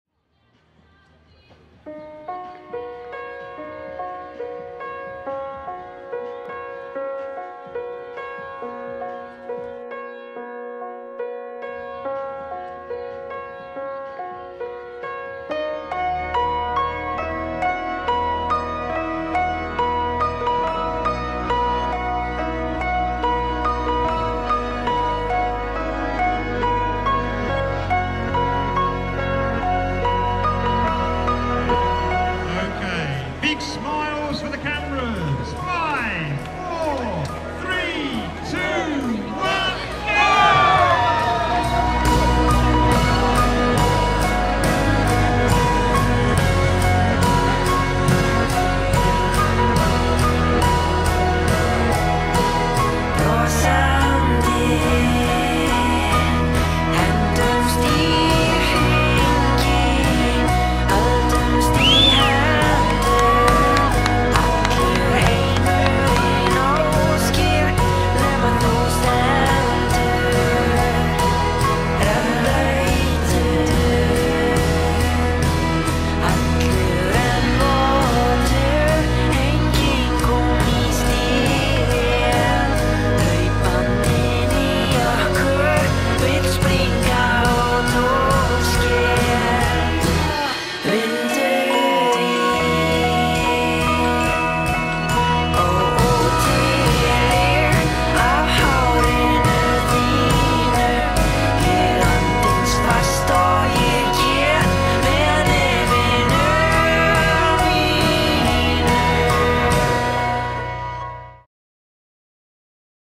Survival of the Fittest 2009 - Caerdydd
Opening of last year's event, 2009, until I fell and broke my arm...and I was only filming it!